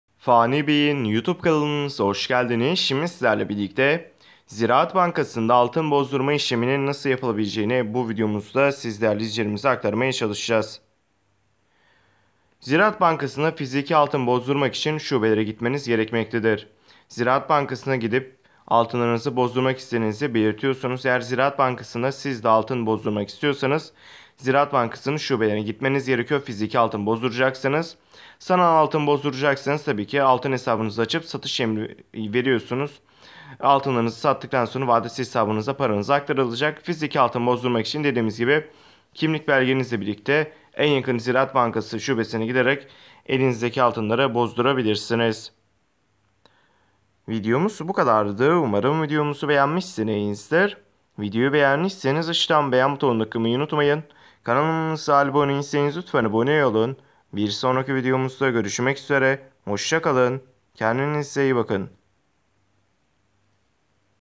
Ziraat Bankası Altın Bozdurma Nasıl Yapılır?
Merhabalar bu videomuzda sizlerle birlikte ziraat bankası altın bozdurma nasıl yapılır sorusuna yanıt vermeye çalışıyoruz.İyi Seyirler Dilerim.

ziraat gram altın bozdurma
ziraat altın nasıl bozdurulur
ziraat altın bozdurma fiyatı